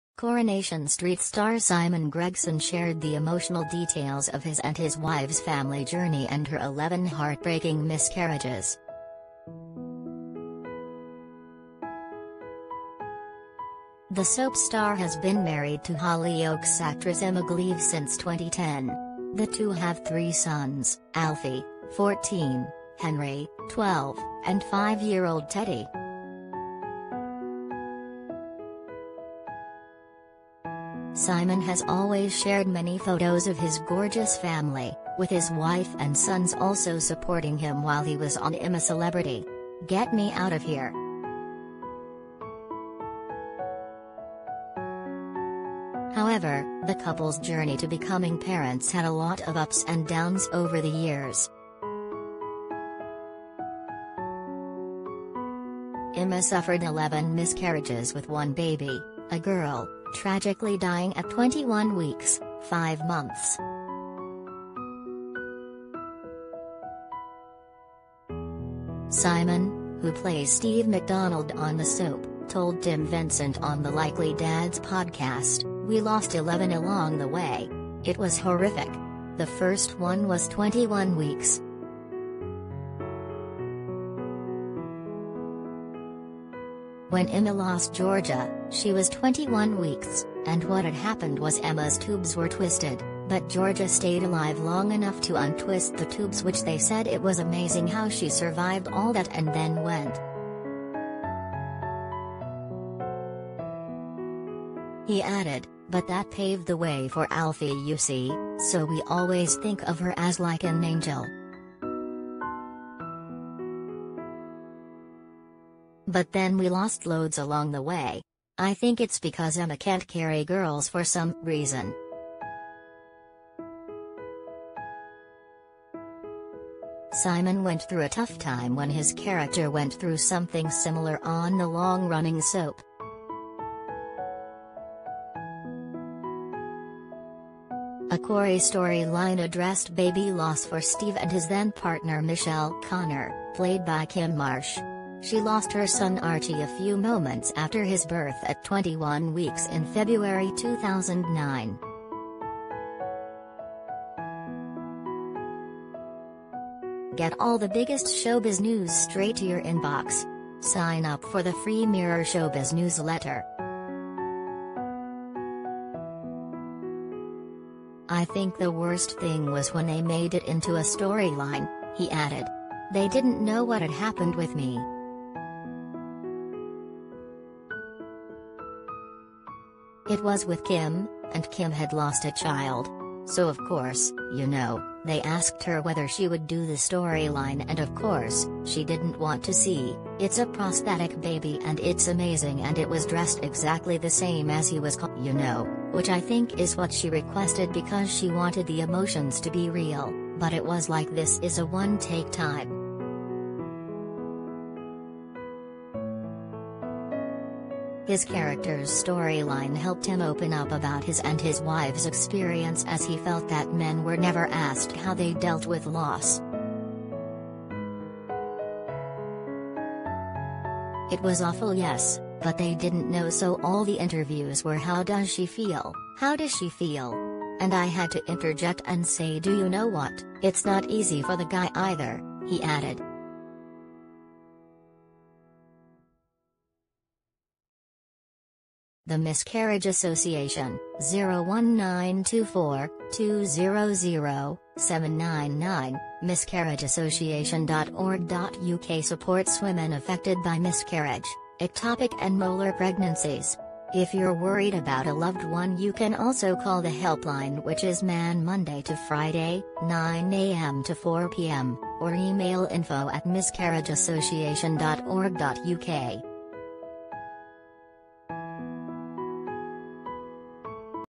Corrie's Simon Gregson says his wife 'lost 11 along the way' to becoming parents
Coronation Street star Simon Gregson shared the emotional details of his and his wife's family journey and her 11 heartbreaking miscarriages.
The soap star has been married to Hollyoaks actress Emma Gleave since 2010. The two have three sons, Alfie, 14, Henry, 12, and five-year-old Teddy.
Simon has always shared many photos of his gorgeous family, with his wife and sons also supporting him while he was on I'm A Celebrity... Get Me Out Of Here!
However, the couple's journey to becoming pare